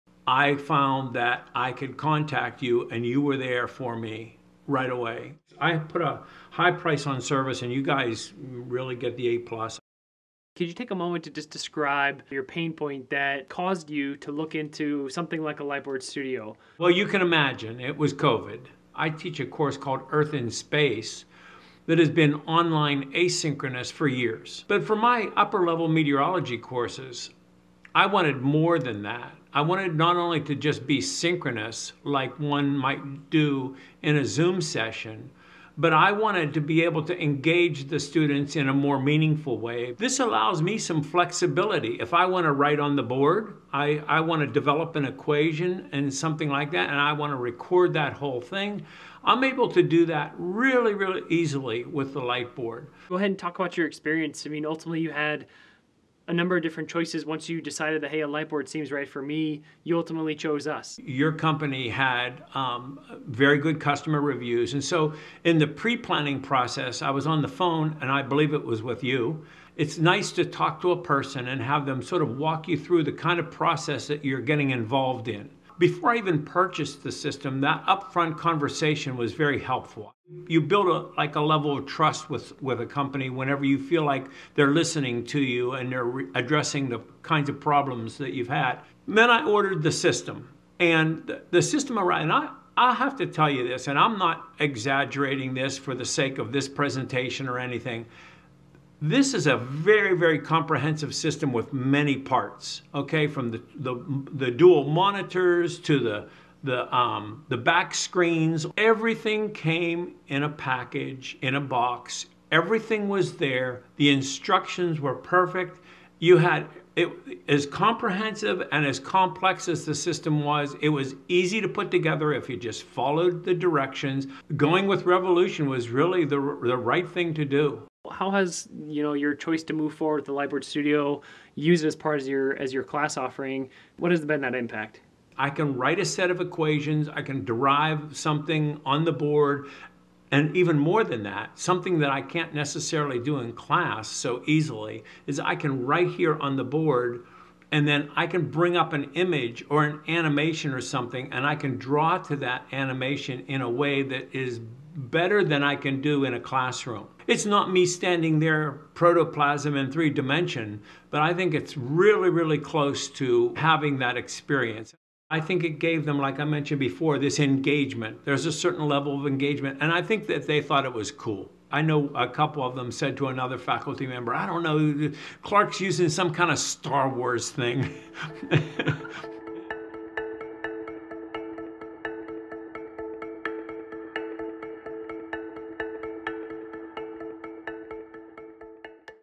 User Story: Richard Clark (Millersville University) & Revolution Lightboards
Dr. Richard Clark is the Chair of the Department of Earth Sciences and Professor of Meteorology at Millersville University. When his courses went fully-remote last year, he found that the lightboard was a perfect solution for synchronous online learning. Richard speaks with Revolution's own Trent Moeller on how the lightboard elevated his online courses.

0:00 - Intro
0:10 - What caused you to research lightboards?
0:52 - Why did you choose Revolution?
1:28 - Describing the comprehensive nature of the system
2:08 - How has the lightboard impacted your teaching?
2:45 - Increased engagement in the classroom.

Find out more about our lightboards, our company, or helpful resources